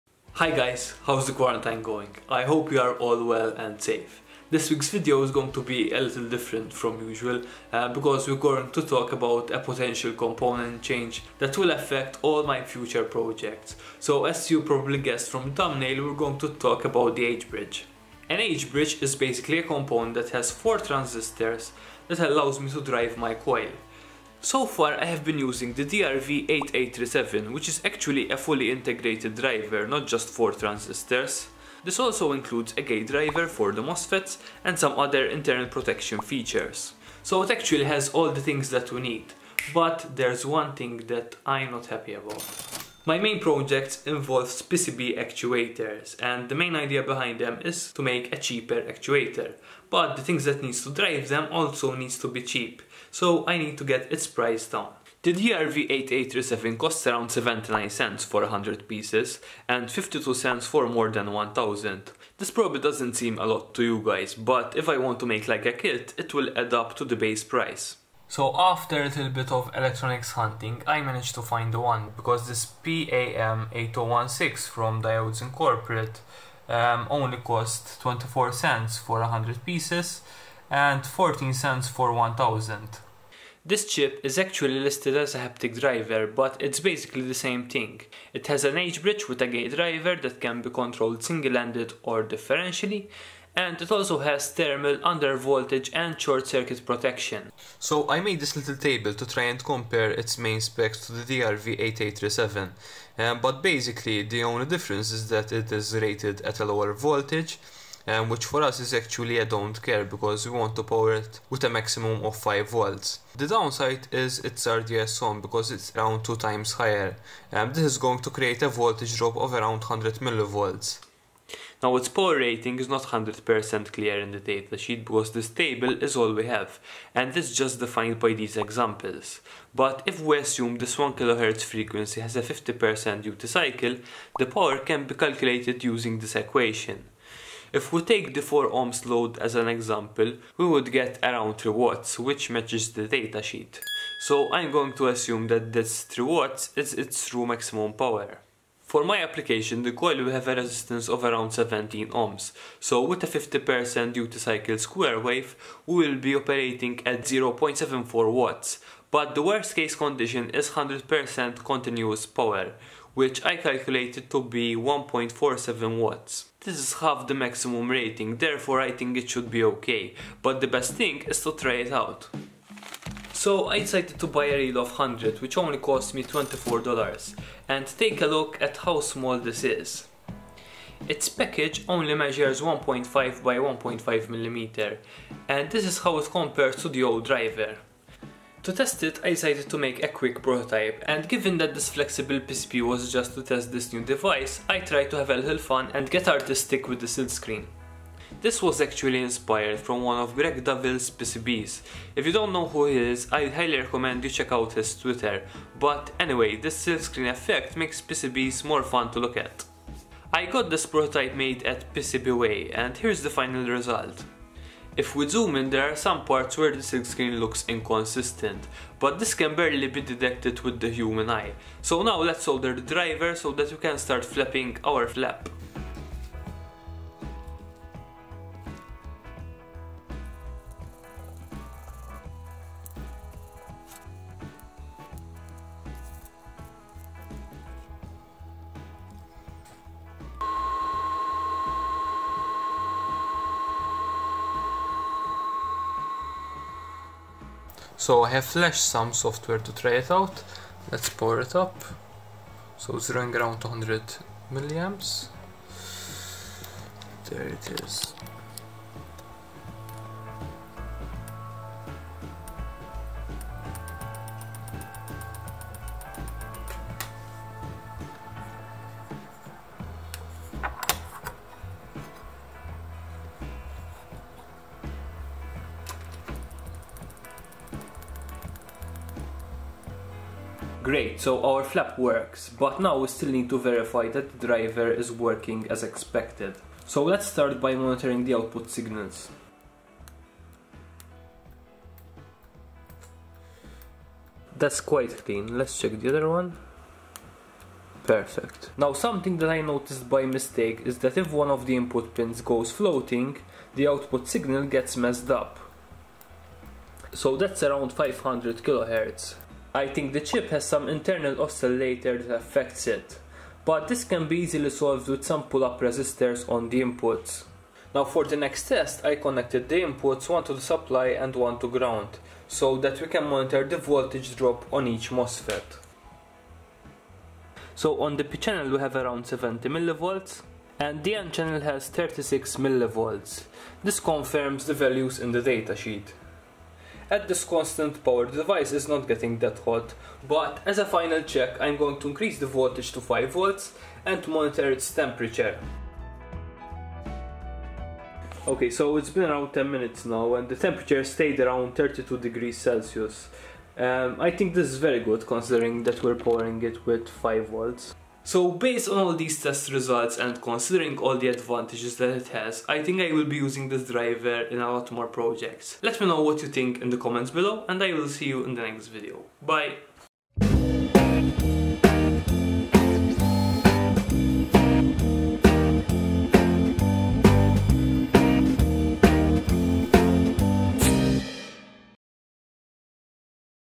Selecting a new H-bridge Driver!
In this video we discuss the SUPER tiny (1.5mmx1.5mm) SUPER cheap (0.14$ cheap (depending on the quantities)) PAM8016 driver and compare it to the DRV8837 (which was the other integrated h-bridge driver that i was using). Thank you PCBWay for sponsoring this project, check out there services

Main Camera
Second Camera
Light Setup
Magnifying Lamp
Vernier Caliper